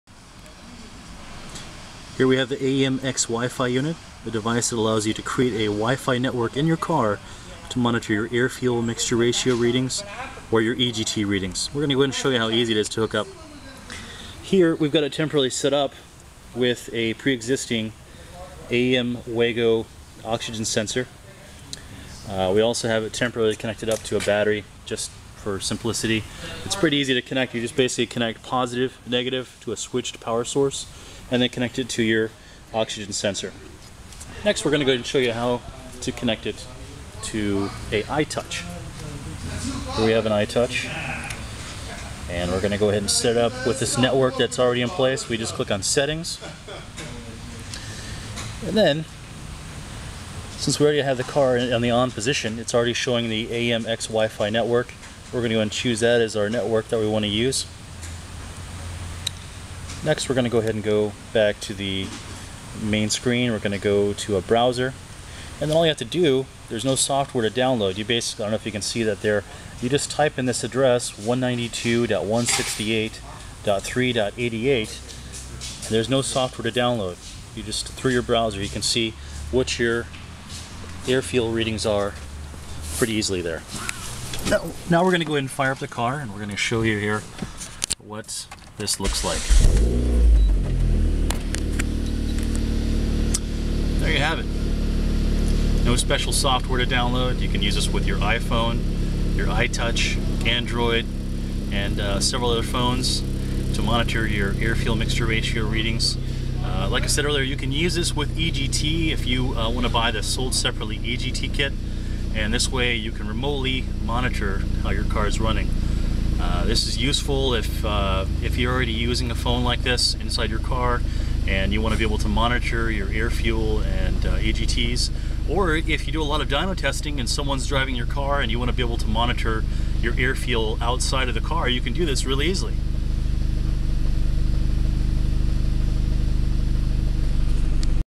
AEM X-WIFI-View your Air/Fuel ratio & EGT on your PHONE !
AEM X-Wifi allows you to create a wireless network in your car to monitor Air/Fuel ratios, Exhaust Gas Temperature (EGT) from your phone! No special software needed! Available at ModernPerformance.com !
Comes with an AEM Air/Fuel WIDEBAND oxygen sensor and wiring (EGT sold seperately) Works with all vehicles. Analog output for datalogging Air/Fuel ratios or for use with EMS. Extended range for wifi - has been tested up to 100 yards away!